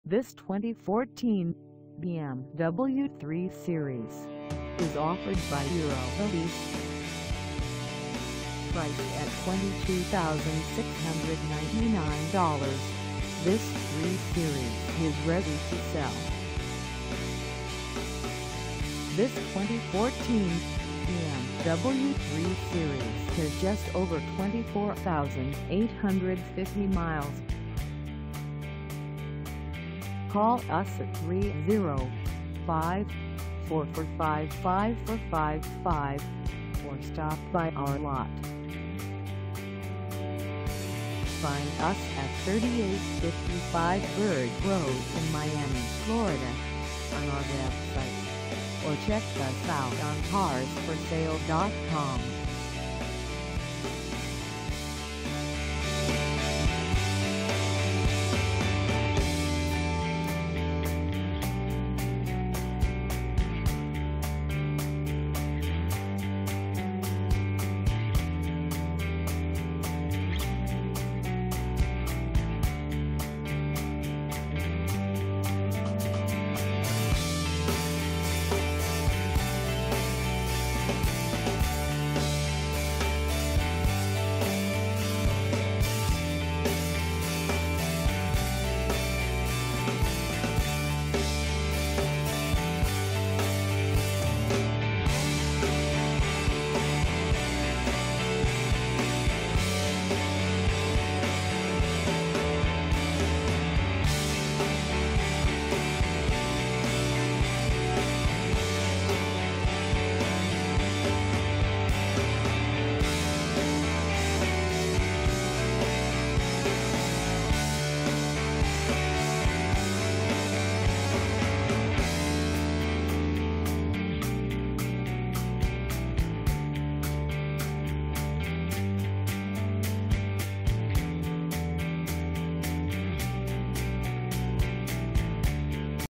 2014 BMW 3 Series 328i 4dr Sedan SULEV for sale in Miami, FL
This 2014 BMW 3 Series 328i 4dr Sedan SULEV is for sale in Miami, FL 33146 at Eurotoys.

Engine: 2.0L I4
Transmission: Automatic 8-Speed
Color: Black Sapphire Metallic
Mileage: 24850
Contact Eurotoys 3855 Bird Road Miami, FL 33146 to test drive this 2014 BMW 3 Series 328i 4dr Sedan SULEV today.